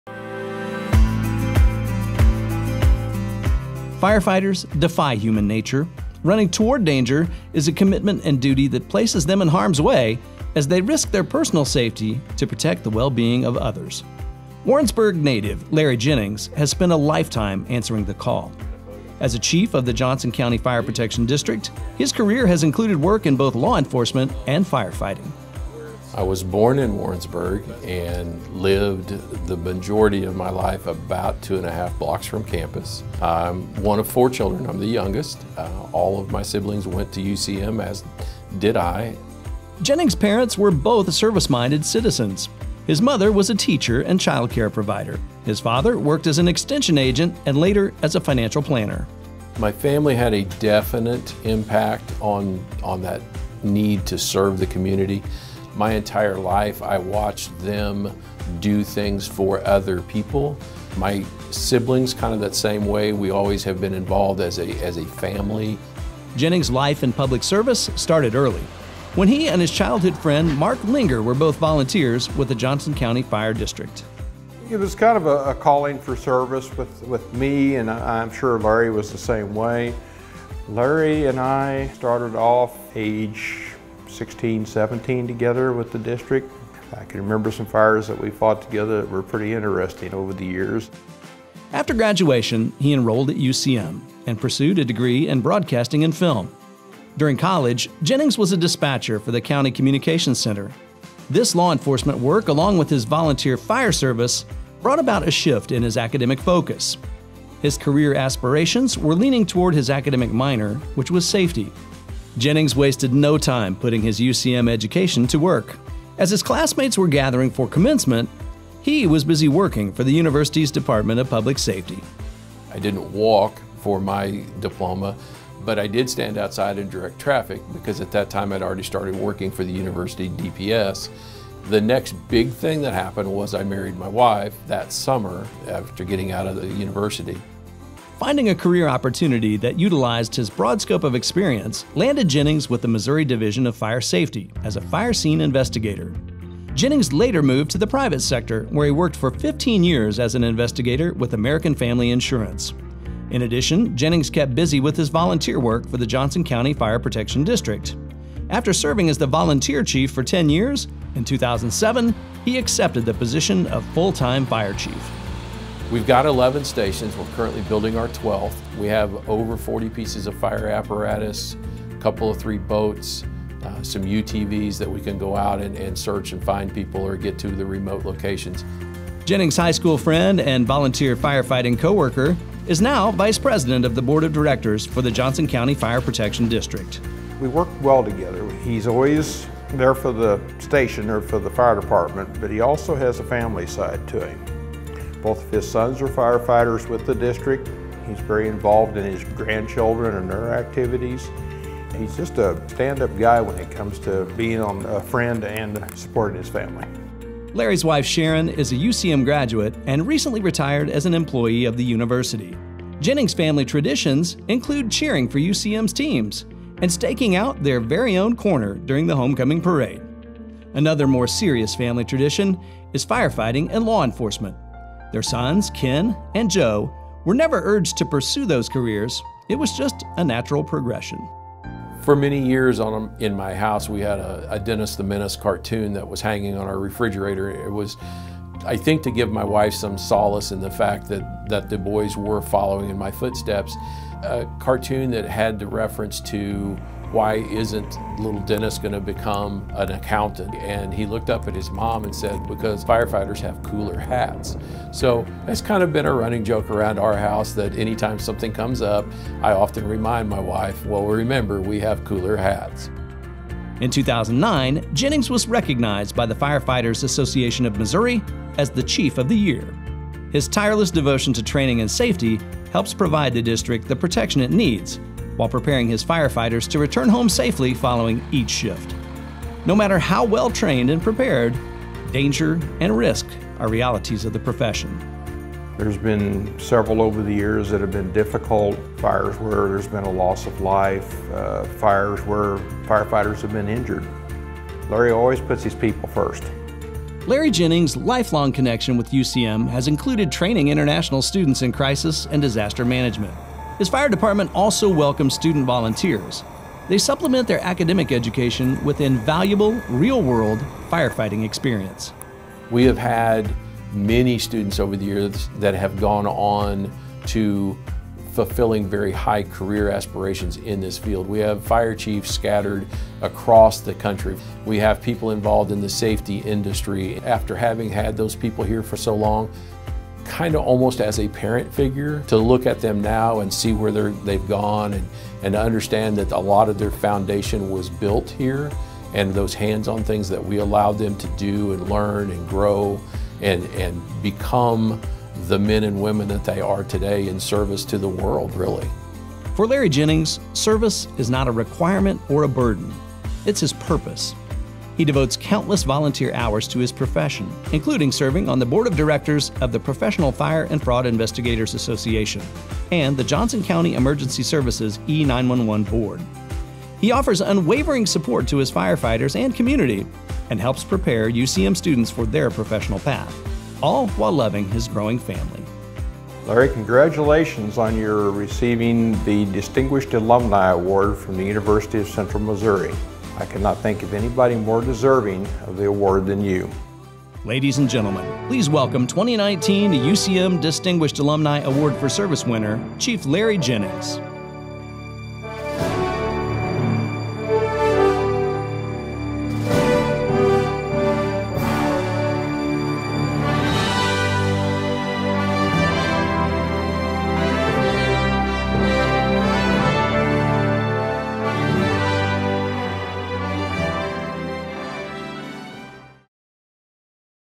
Larry Jennings - UCM Distinguished Alumni 2019
Longtime Warrensburg resident Larry Jennings earned his bachelor’s degree in broadcast media with a minor in safety from UCM. He cites his family’s involvement in the community as “probably the biggest influence” on his decision to pursue a career in service.

Jennings’s interest in firefighting began in 1976 as a volunteer for the Johnson County Fire Protection District. He was a fire scene investigator from 1987 to 1992 for Missouri’s Fire Safety Division. He then spent 15 years as an investigator for American Family Insurance’s special investigations unit. Jennings has been the fire chief of the Johnson County Fire Protection District 1 in Warrensburg since 2007. In 2009, the Fire Fighters Association of Missouri named him Chief of the Year, and he currently serves as the organization’s first vice president. Jennings also serves as director for the Professional Fire and Fraud Investigators Association.

Top priorities throughout his career have included fire suppression and fire safety training for firefighters and residents, resulting in a Johnson County Training and Safety Division and the purchase of a fire and weather safety trailer used for community education. Jennings makes numerous presentations for UCM and has trained and mentored students, volunteers, interns and student residents for decades.